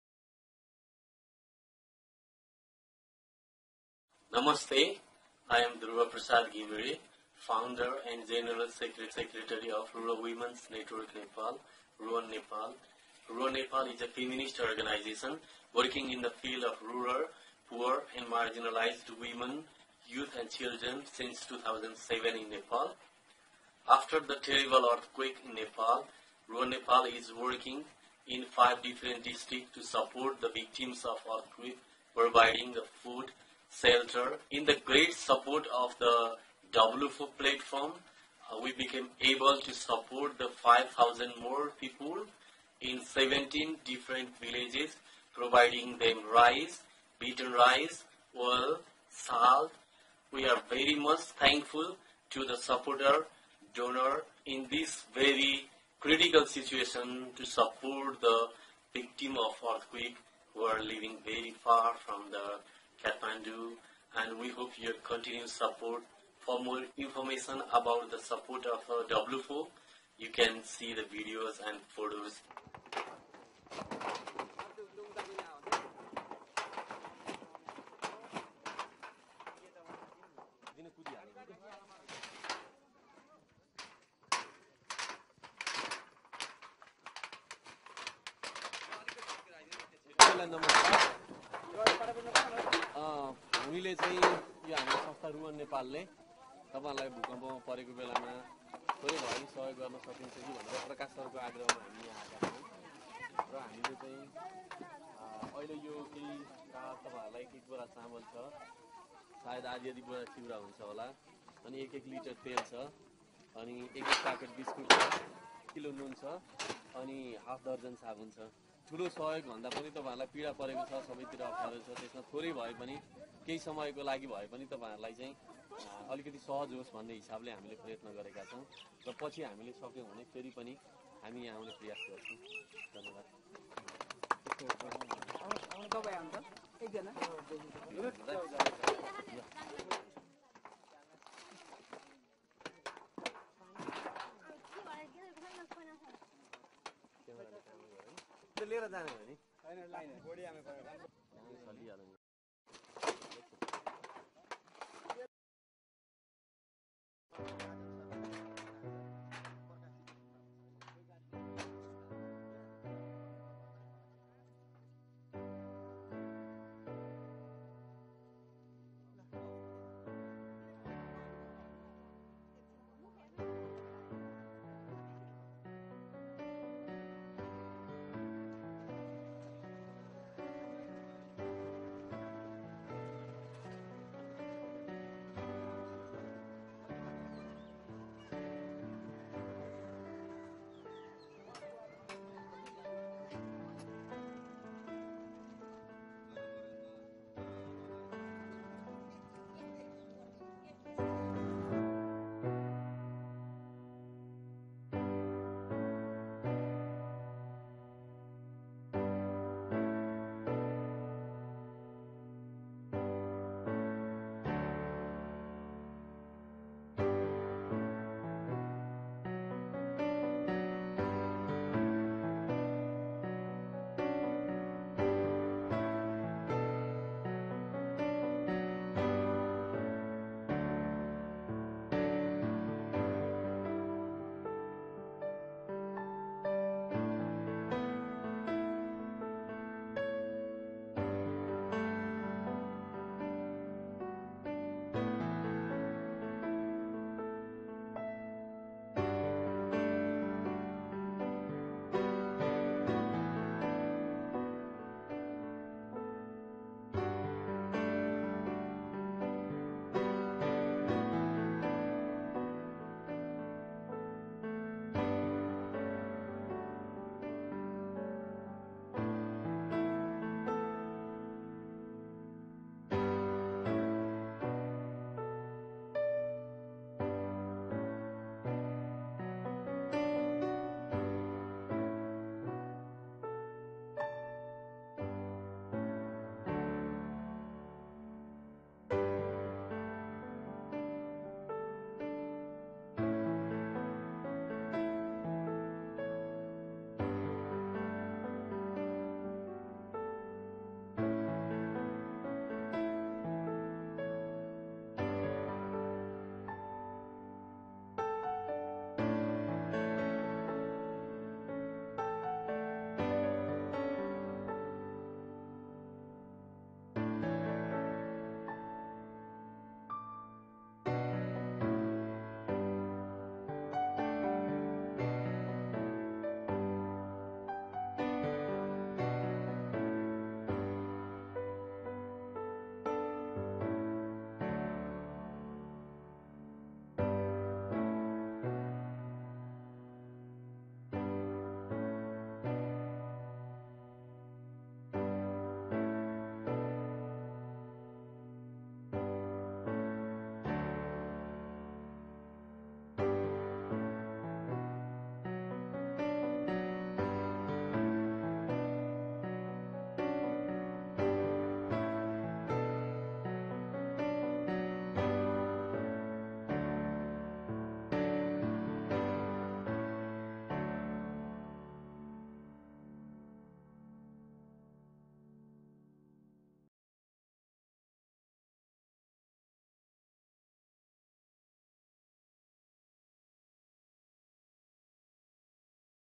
Emergency relief Nepal
Help provide urgent emergency assistance for victims of the earthquake in Nepal